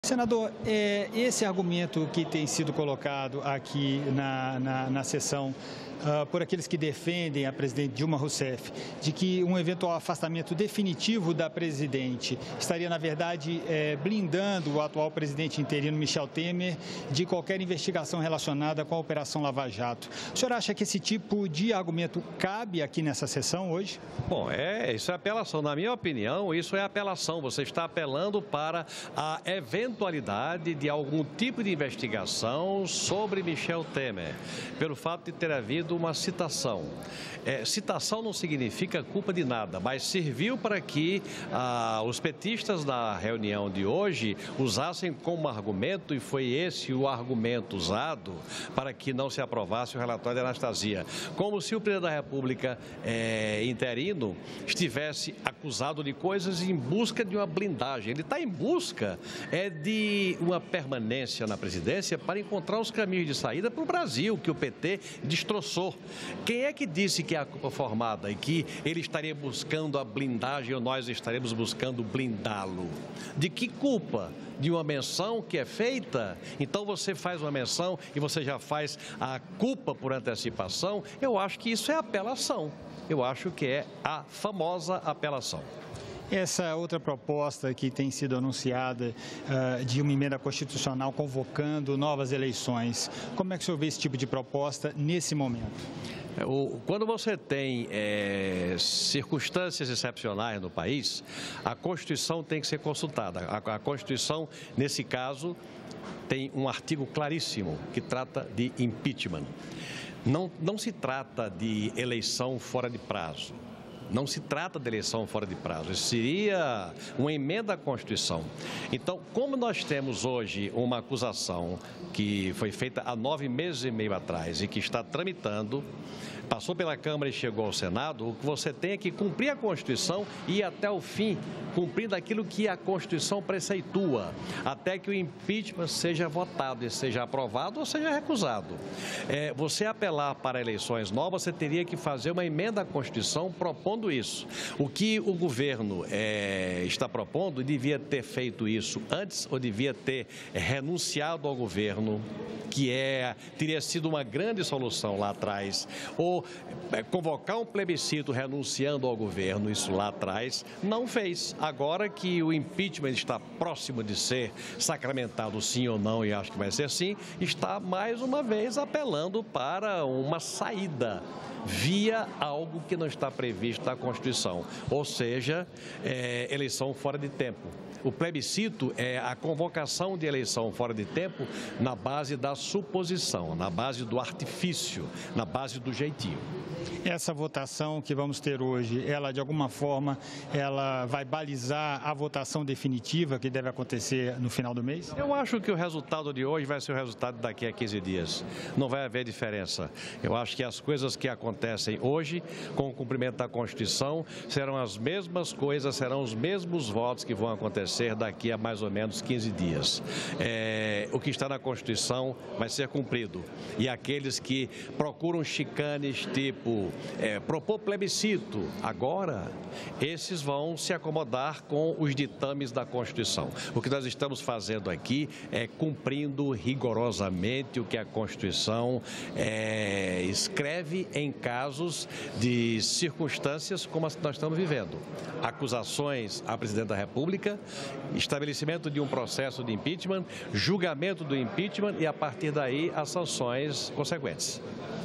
José Agripino chama argumento de senadores da oposição de "apelação"
Senador José Agripino (DEM/RN) argumenta que a citação do nome do presidente interino Michel Temer em denúncias de corrupção serviu como argumento dos senadores da oposição para propor a rejeição do relatório de pronúncia do senador Antonio Anastasia